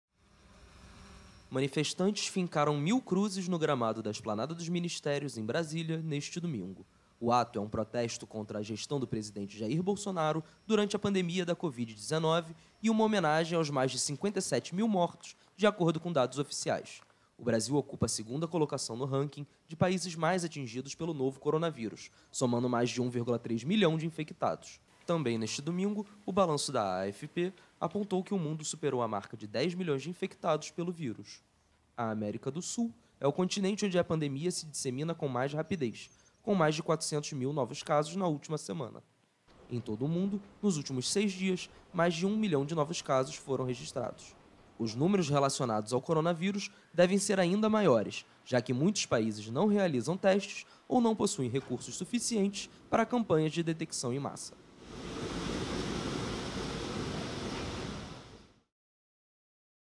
Cruzes em Brasília | AFP
Uma manifestação na Esplanada dos Ministérios, em Brasília, neste domingo, espalhou mil cruzes pelo gramado do local. No mesmo dia, o mundo ultrapassou a marca de mais de 10 milhões de infectados pela Covid-19.